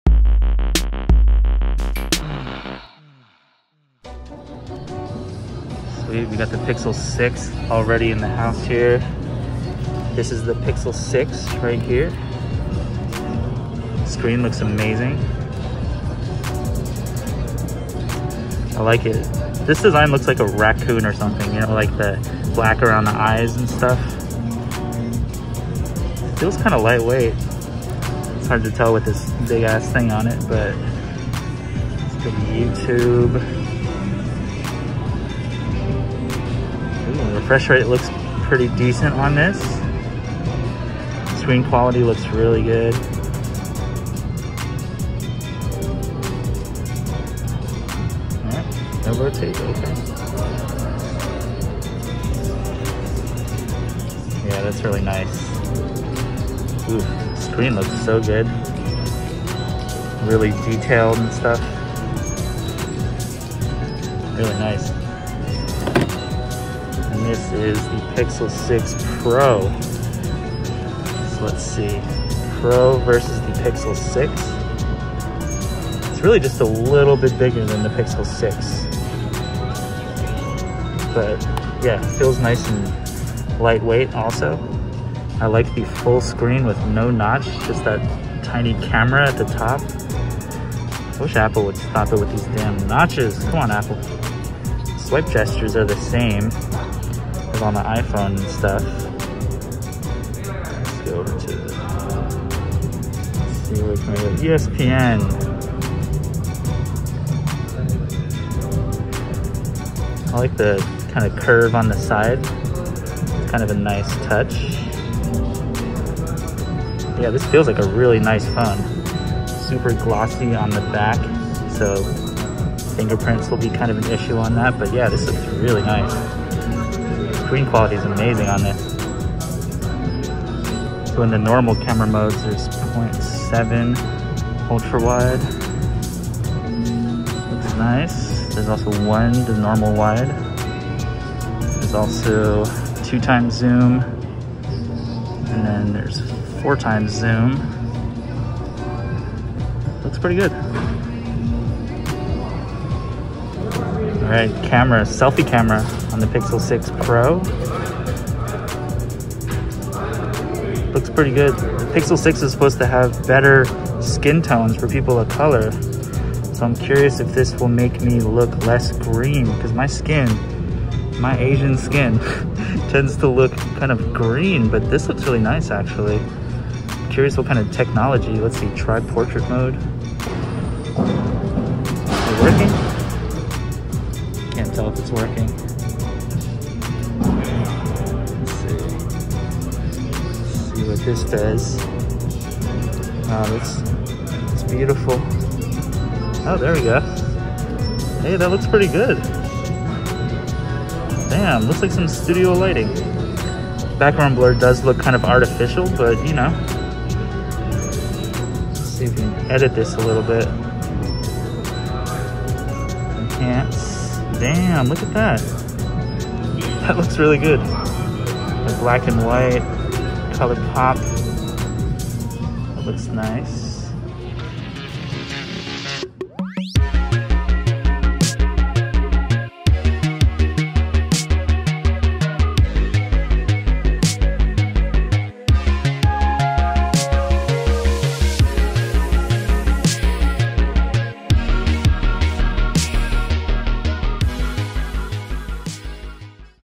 Pixel 6 & 6 Pro at Best Buy!
Pixel 6 Pro

I checked out Google's new Pixel 6 and 6 Pro launch at Best Buy! Here's a really quick look at this awesome new Android phone. I tried the cameras out and they look really spicy 🌶 the Pixel 6's Real Tone feature is advertised to have AI that provides "more equitable camera and imaging products for people of color". It actually made my azn skin look more natural and less green in portrait mode. Miracles 😭

Shot on iPhone 13 mini in 4K Dolby Vision HDR, another incredible and cute af phone.

0:00 Pixel 6
1:06 Pixel 6 Pro
2:11 6 Pro camera demo

pixel6 pixel6pro googlepixel google madebygoogle android teampixel bestbuy android shotoniphone iphone13mini BenAqua AQUAFAM ok real talk the pixel 6 is one of the 1st android phones that has actually made me question whether or not I should switch from iPhone 13 pro 🤐🧐 hmmmmm what will happen what is real what is fake is this all a simulation maybe yes maybe no maybe huh what